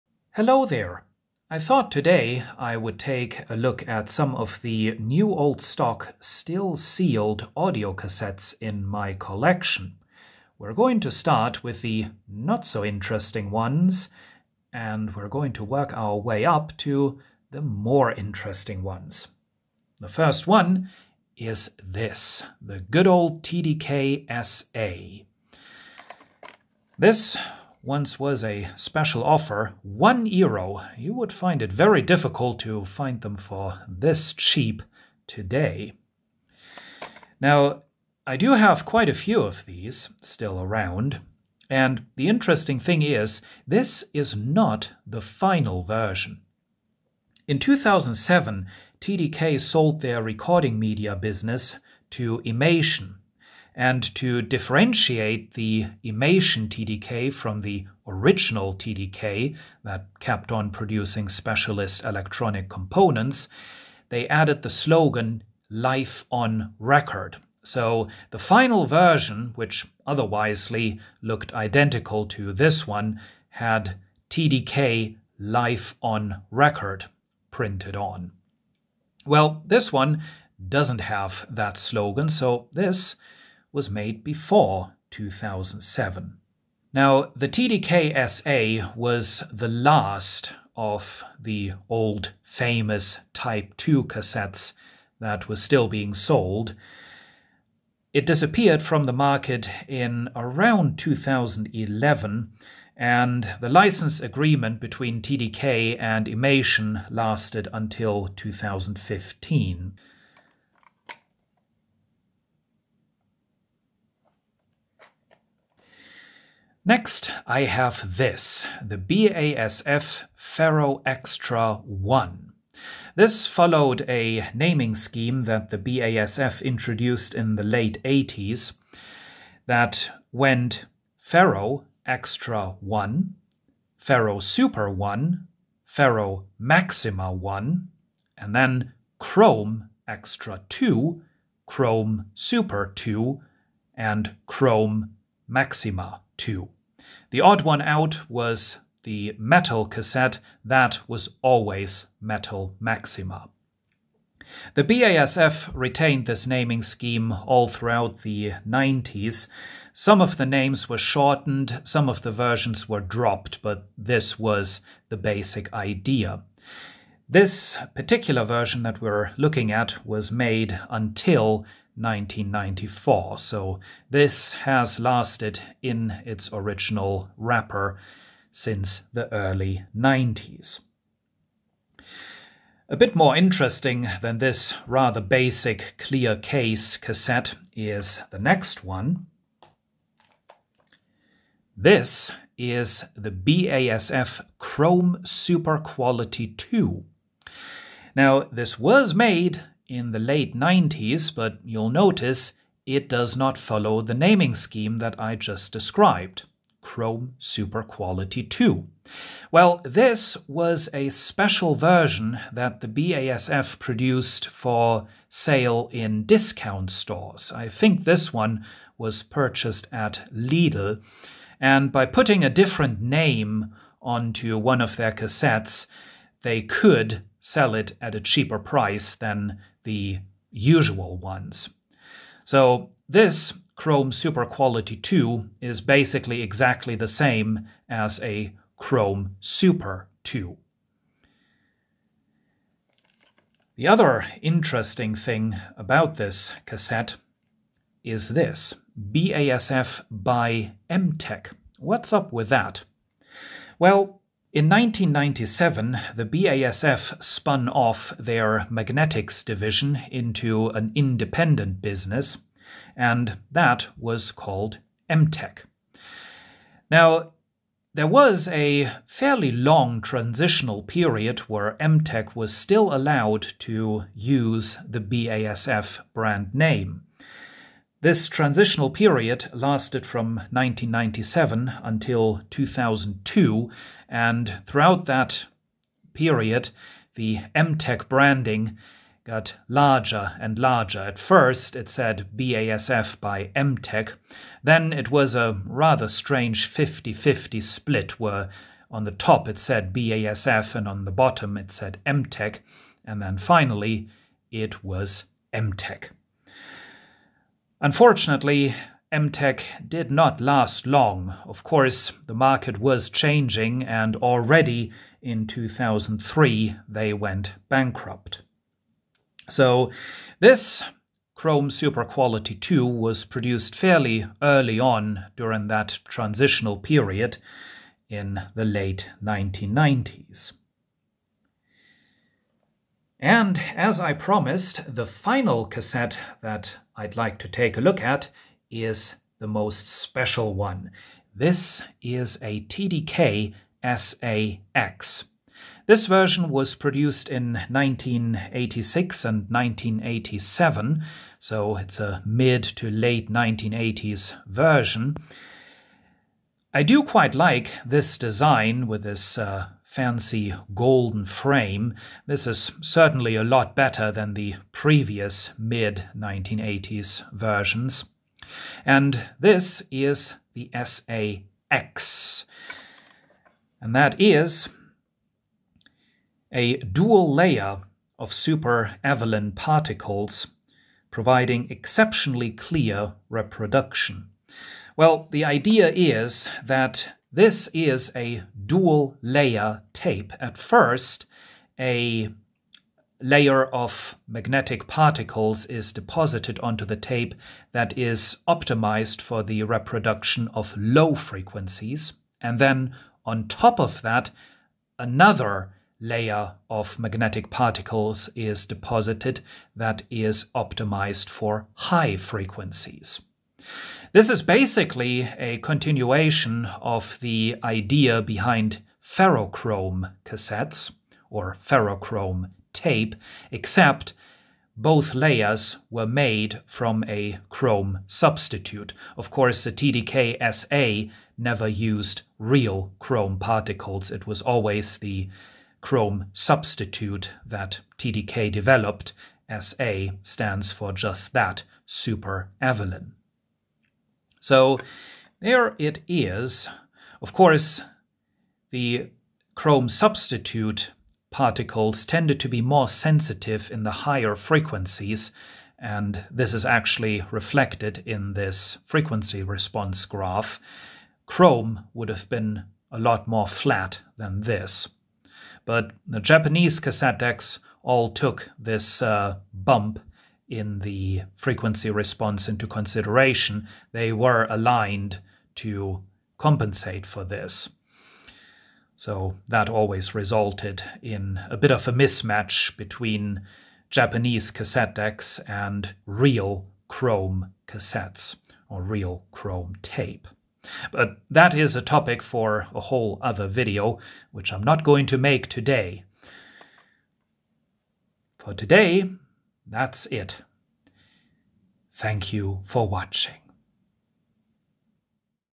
A look at new old stock sealed cassettes in my collection
Taking a look at some new old stock still sealed audio cassettes in my collection.